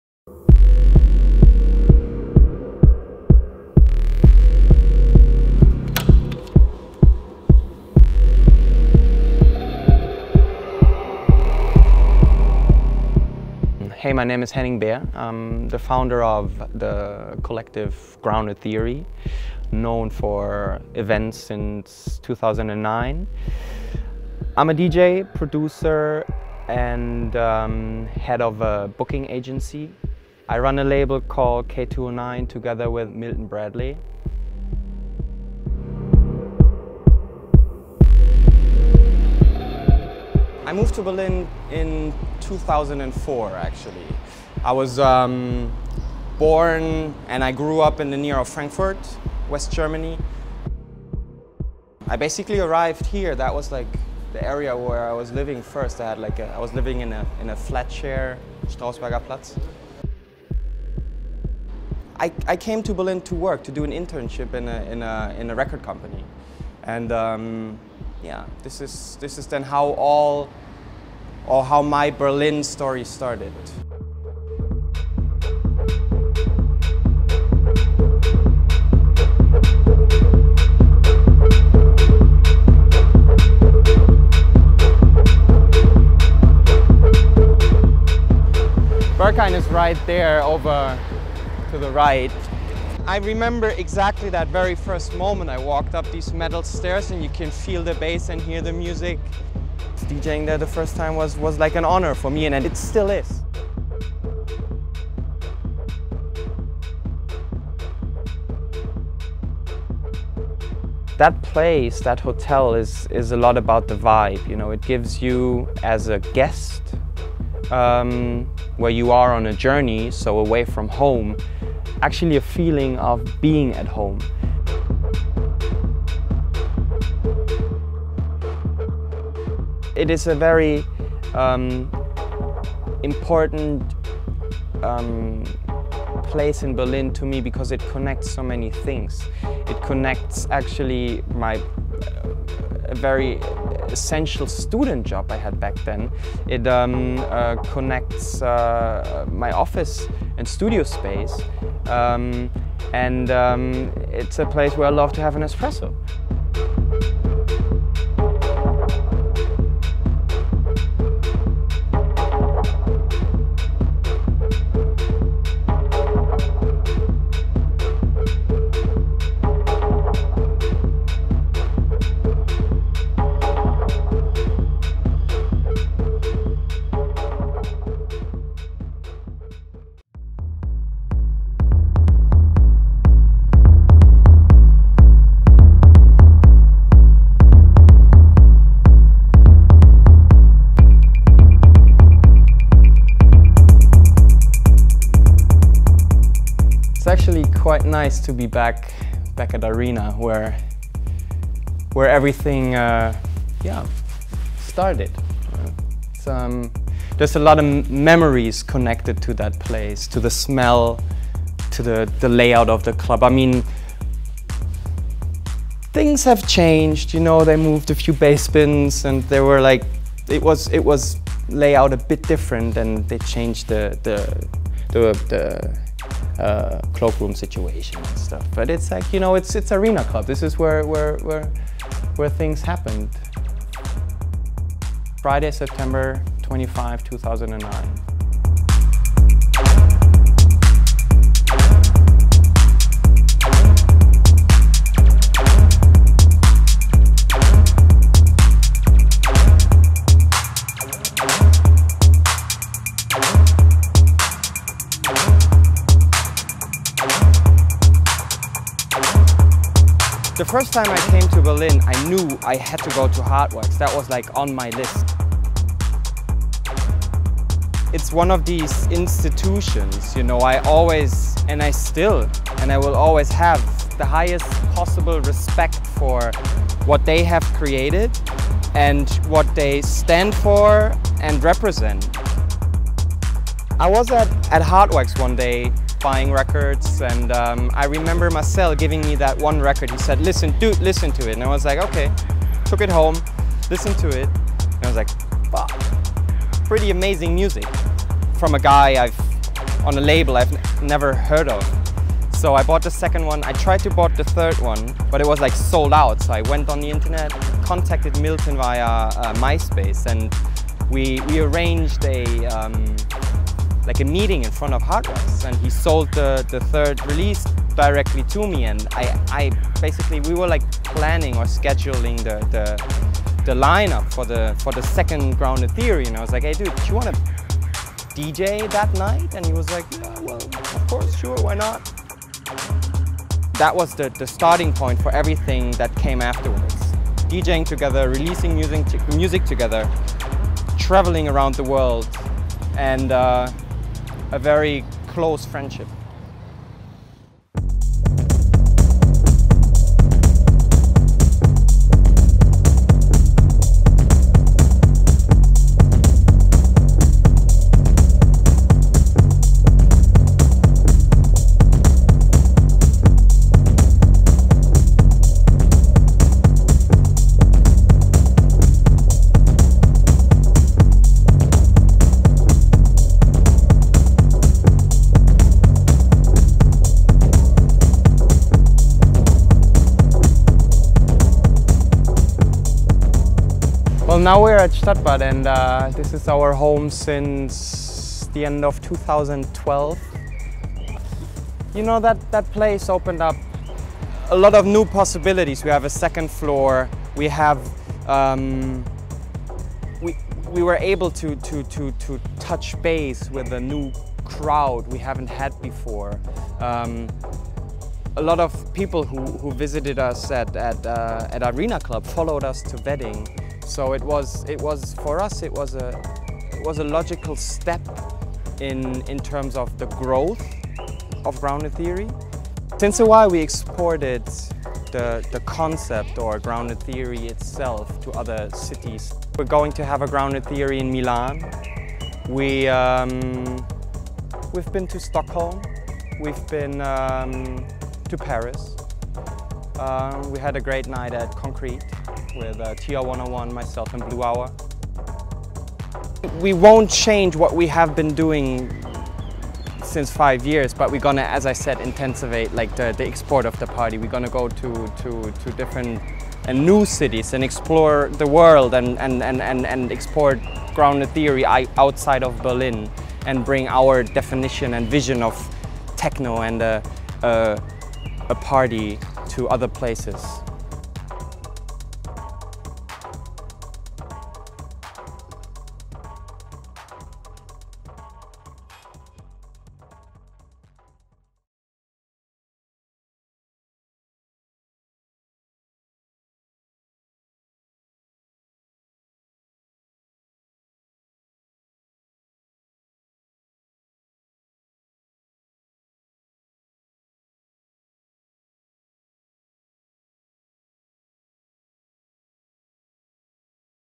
Tales from the Underground: Henning Baer
Henning Baer has grown to become a cult figure within the global techno circuit. Having established the Grounded Theory events in Berlin, the German producer went on to set up the K209 imprint alongside fellow cohort Milton Bradley. Grounded Theory is now a global events brand and booking agency, Baer himself, an in demand techno-star. In the first on our online documentaries in 2015, DJBroadcast tells the story of Baer’s rise to success, through the context of his neue heimat  - Berlin.

Tales from the Underground documents Baer’s chronology through one of Berlin’s most iconic focal points, the UBahn. As an integral part of the city’s present day story and history, the UBahn has connected people from all across the city for over a century, whether taking people to work, or to their favourite nightspots.

Join us as we follow Henning Baer across Berlin, as he details his relationship with Berlin, from where he first lived, to the clubs he’s played in, to the areas that mean the most to him.